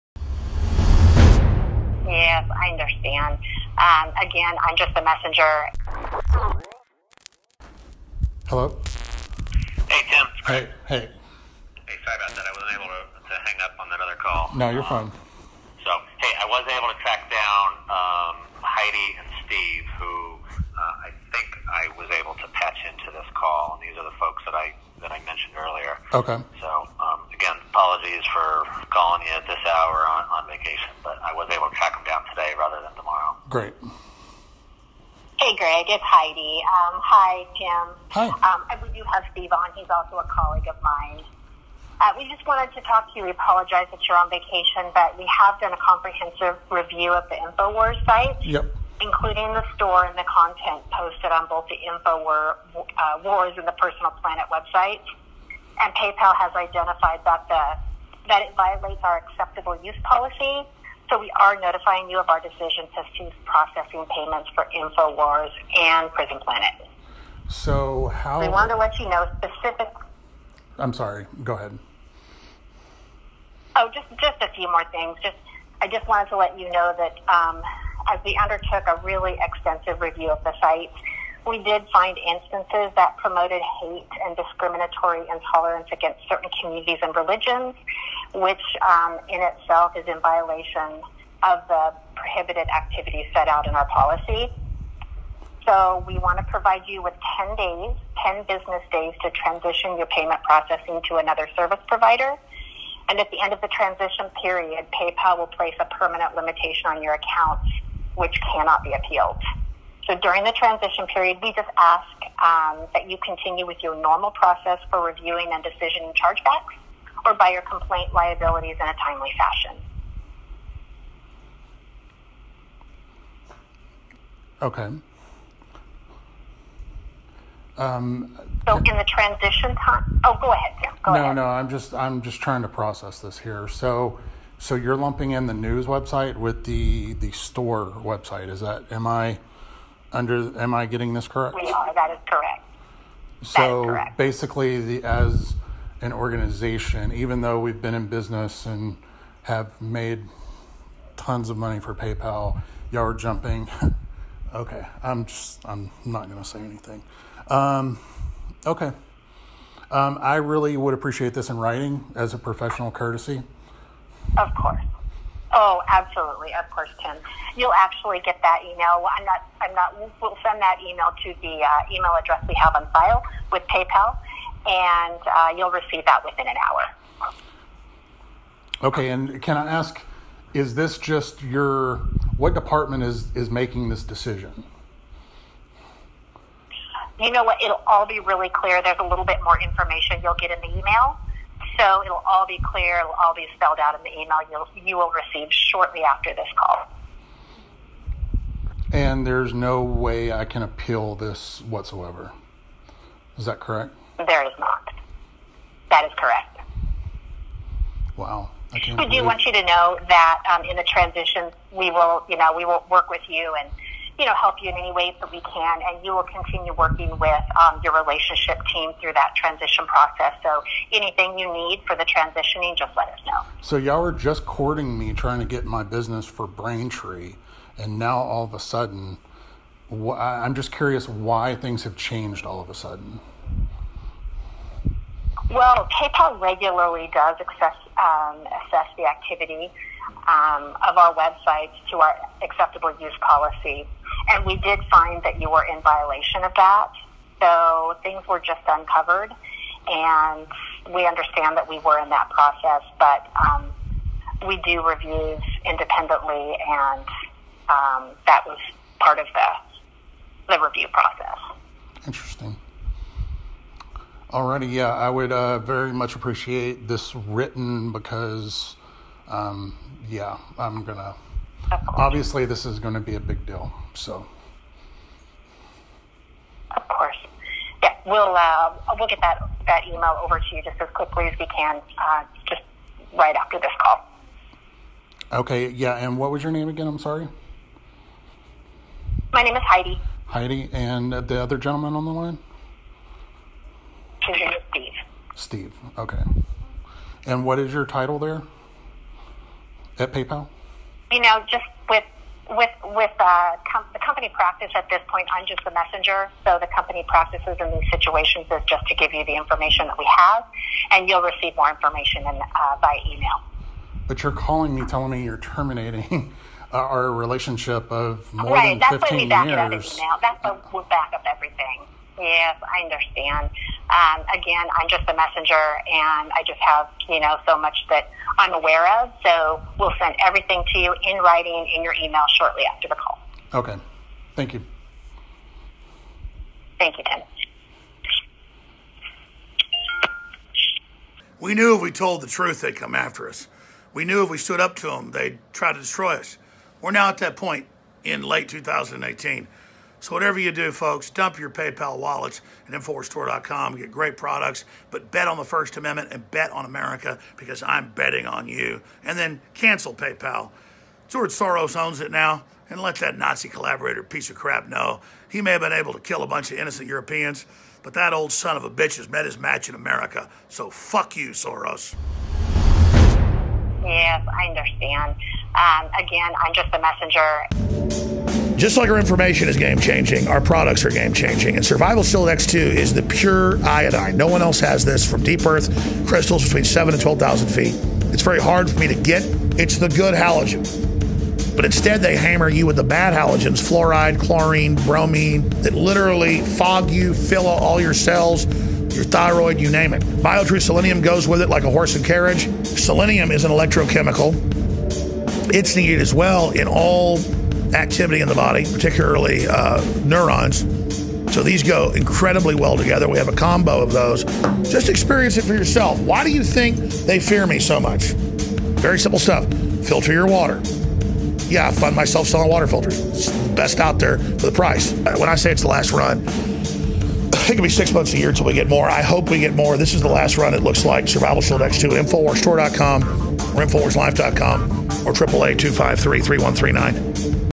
Paypal BANS Alex Jones and Infowars | Phone Call of No Longer Being Pals
What's next for Alex Jones and the crusade against him? His mom? Maybe his dog will disown him? If this is what the future looks like for truth seekers, then maybe the world will become crypto centric sooner rather than later.

This video, Shock Audio: Listen to the Phone Call where PayPal Banned Infowars, was uploaded by Alex Jones' Therapist and was provided to A Billion Views via creative commons.

Alex Jones' Therapist's comments below